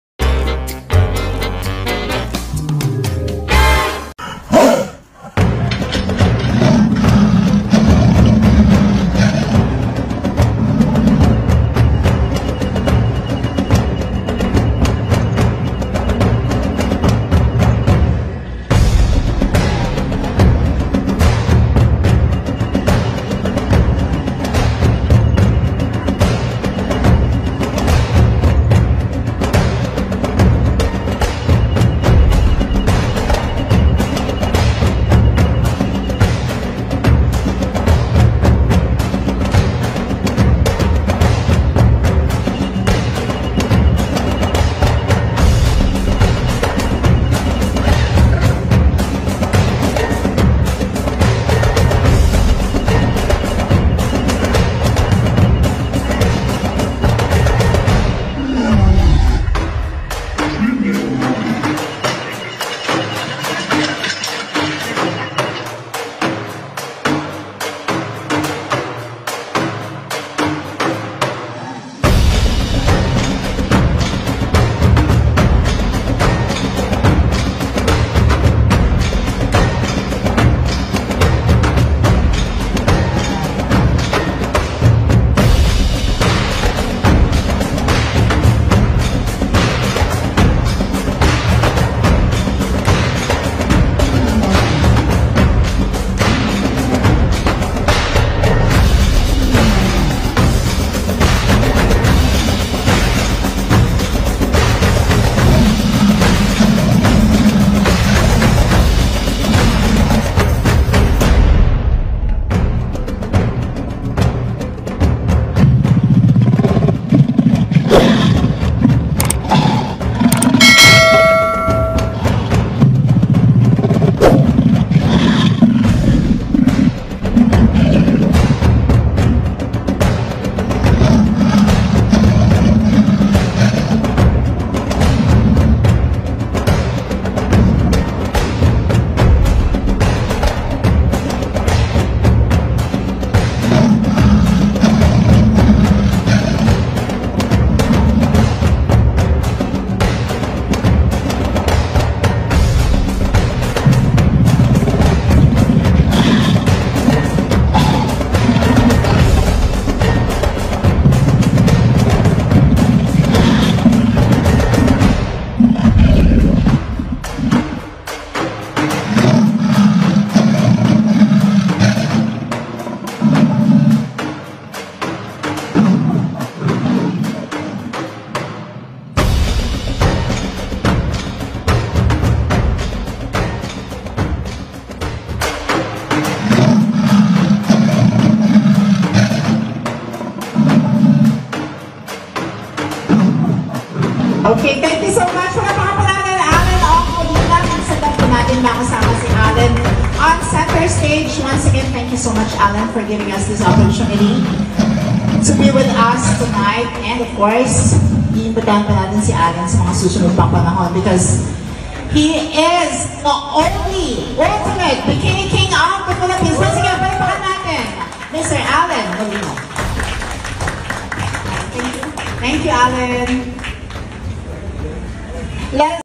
LOOK! HOTNESS OF ALLEN ONG MOLINA I THE ULTIMATE BIKINI KING OF THE PHILIPPINES
Allen Ong Molina flexing his body who's been considered as the Bikini King of the Philippines.